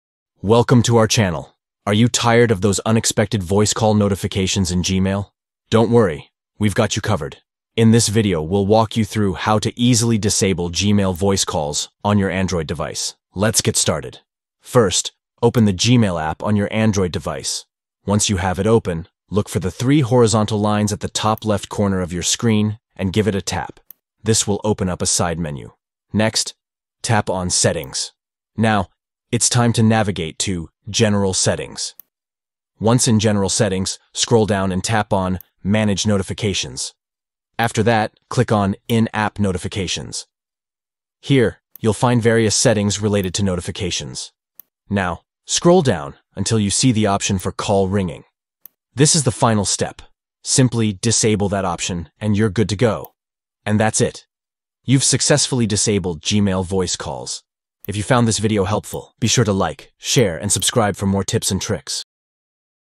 How to Disable Gmail Voice Call (Updated)
Learn how to disable Gmail voice call notifications effortlessly in this updated guide! Whether you find the notifications disruptive or simply prefer a quieter inbox, we've got you covered. Follow these simple steps to take control of your Gmail app on Android

Steps:
1. Open the Gmail app on your Android device.
2. Tap the three horizontal lines at the top left.
3. Tap "Settings".
4. Tap "General Settings".
5. Tap "Manage notifications".
6. Tap "In app notifications".
7. Scroll down then Disable Call ringing.

Chapters:
00:00 Intro
00:12 Soultion
01:05 Outro